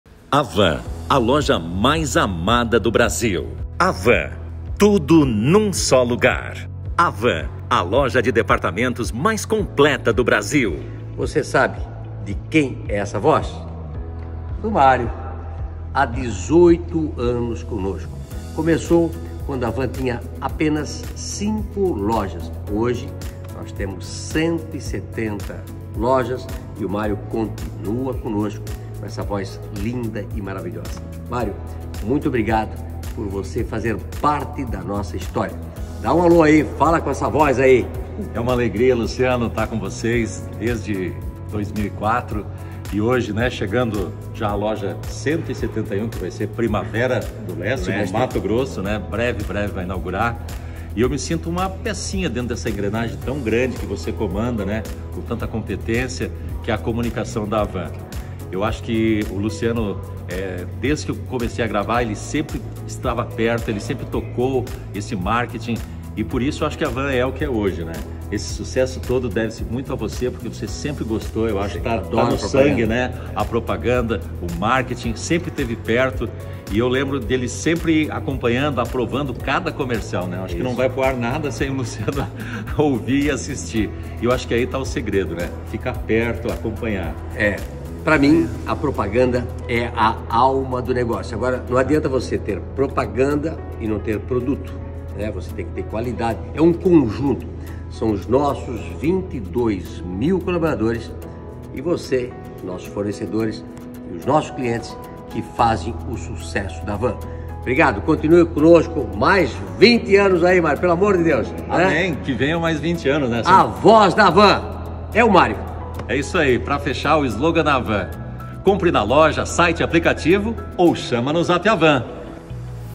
CONHEÇA A VOZ DA HAVAN | LUCIANO HANG
Quem acompanha os comerciais da Havan já deve ter reparado que há muitos anos a voz que narra nossos materiais é a mesma. Tive o grande prazer de receber o Mario Roberto Beling no nosso administrativo, a “voz da Havan” há 18 anos. Uma pessoa maravilhosa e nosso parceiro do coração. Como é bom saber que ao longo desses quase 36 anos de empresa construímos histórias lindas com colaboradores, clientes e fornecedores. Que possamos seguir assim, encantando a todos. O Mario compartilhou algumas lembranças e comprovou para as novas gerações do marketing Havan que eu sou “chato” desde sempre, estou sempre me envolvendo e opinando. Confira no vídeo!